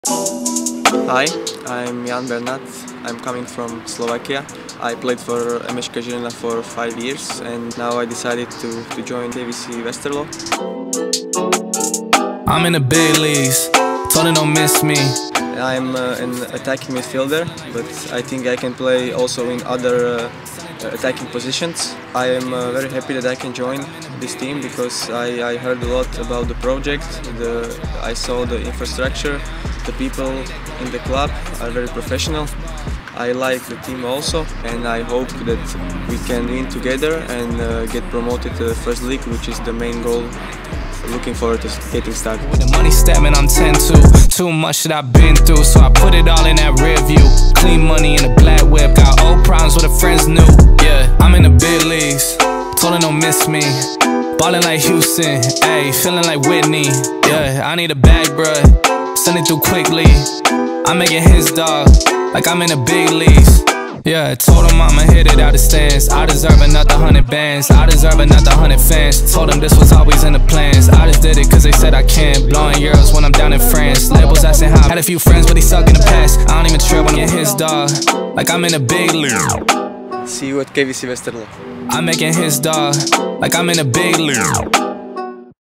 Welkom Ján Bernát
KVC Westerlo heet Ján Bernát van harte welkom bij de club.

KVC Westerlo warmly welcomes Ján Bernát to the club.